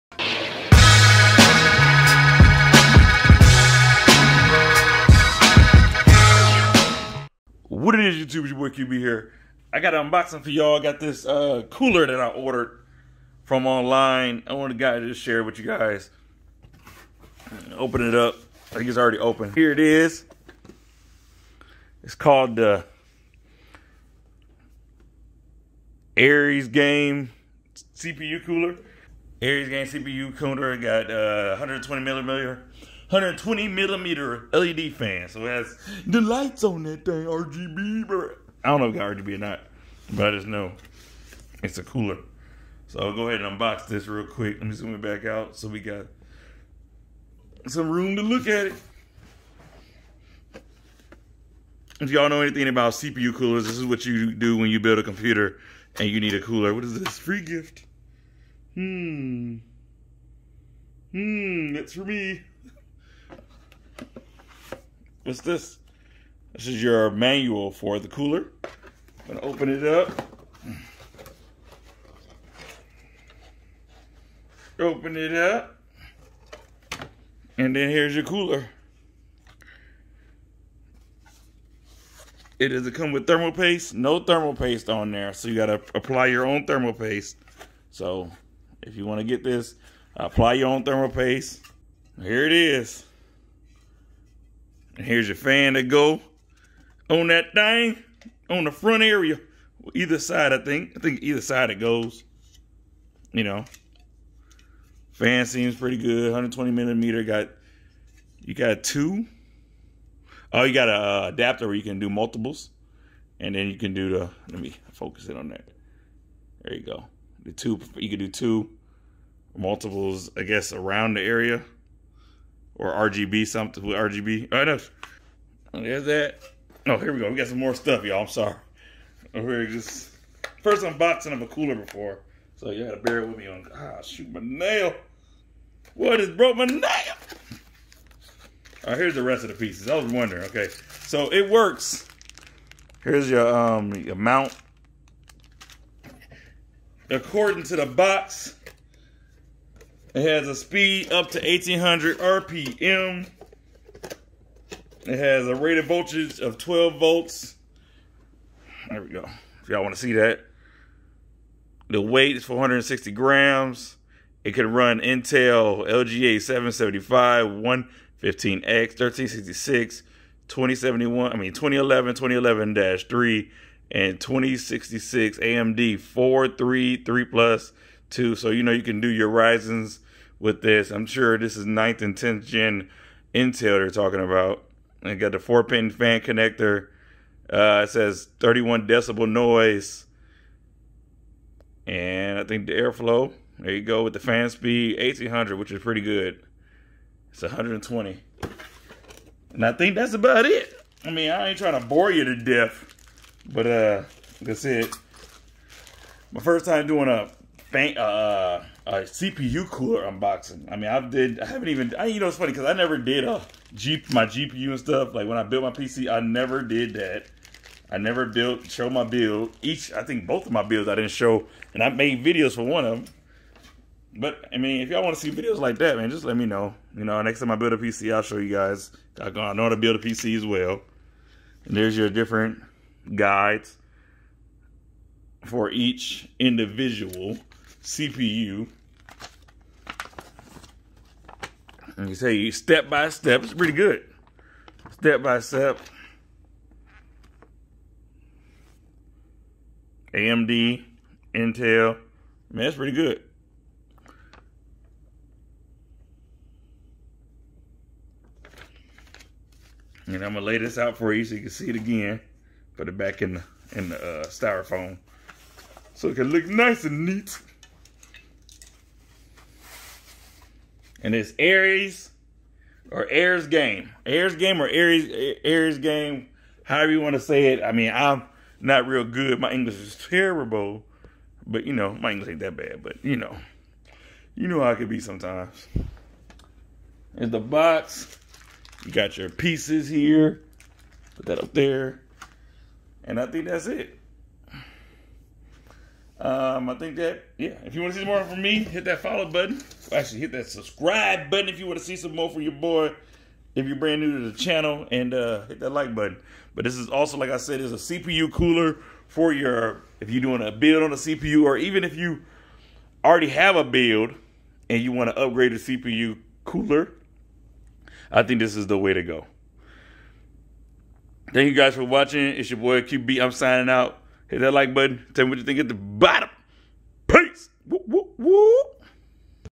AresGame River 5 CPU Cooler Unboxing
Check out AresGame Cpu Cooler as I unbox it first hand. I didn't get a change to try it out because I don't have a build ready for installation.